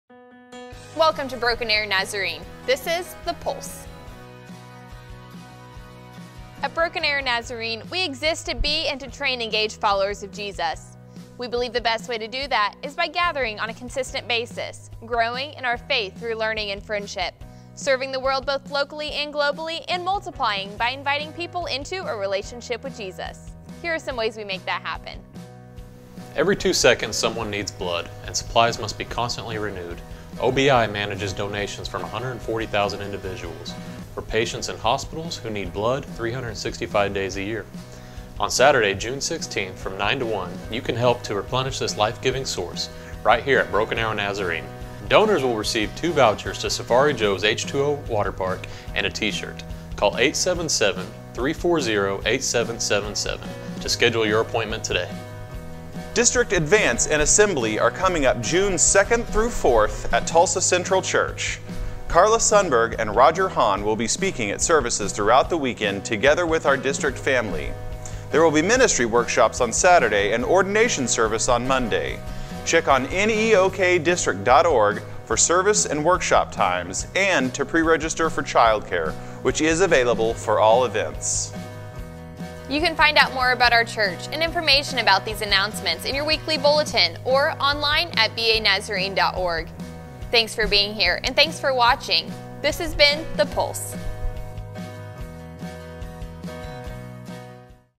The Pulse 052718
Weekly Announcements for May 20, 2018:
District Advance & Assembly - June 2-4
OBI Blood Drive -  June 16
For more information about our church visit banazarene.org